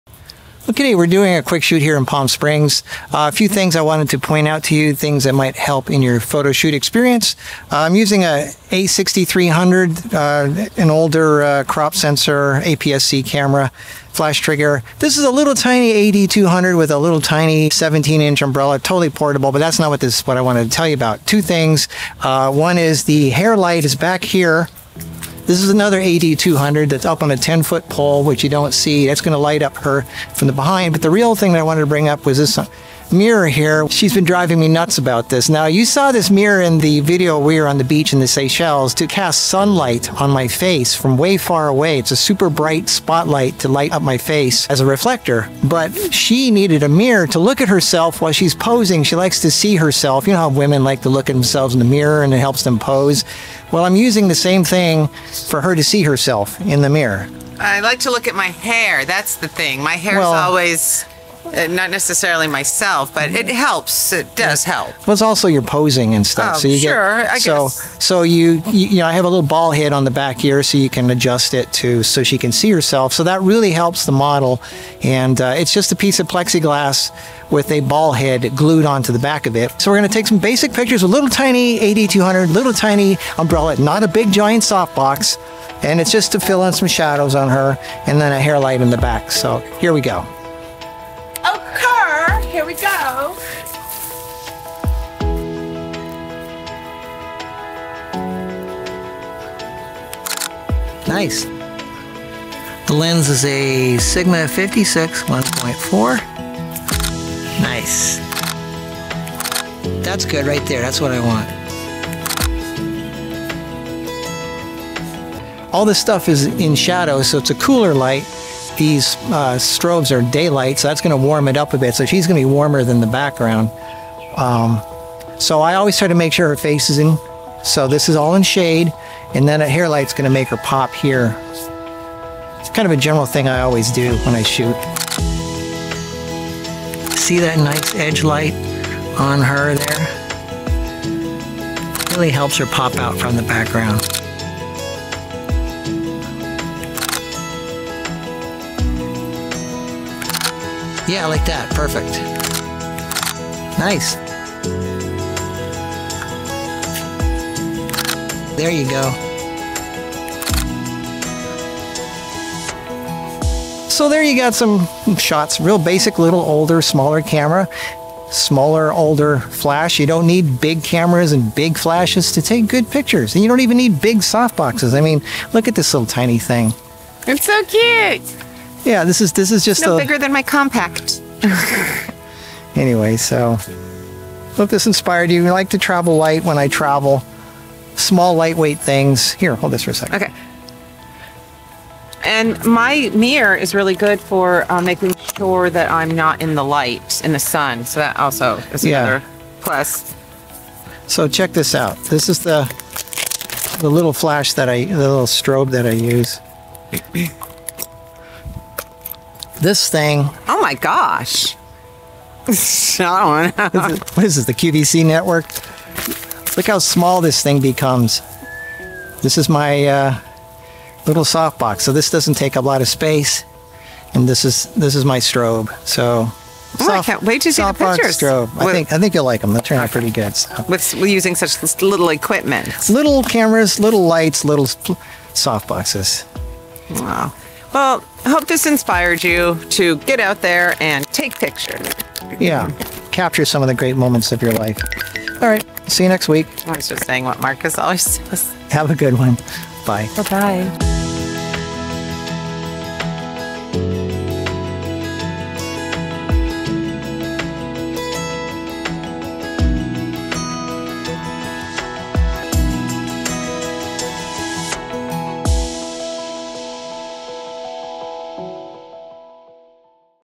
Using a Mirror for Outdoor Portrait Photography with little flash in shade
Here's a photography tip to help your model posing- have a mirror on a light stand so she can see herself. It really helps her know what's going on, resulting in better poses, expressions and shots.
gear used
Godox AD200 portable flash strobe B&H
Godox AD200 portable flash strobe Amazon
Sony A6300 APSC camera
Godox XPro-S TTL Wireless Flash Transmitter Trigger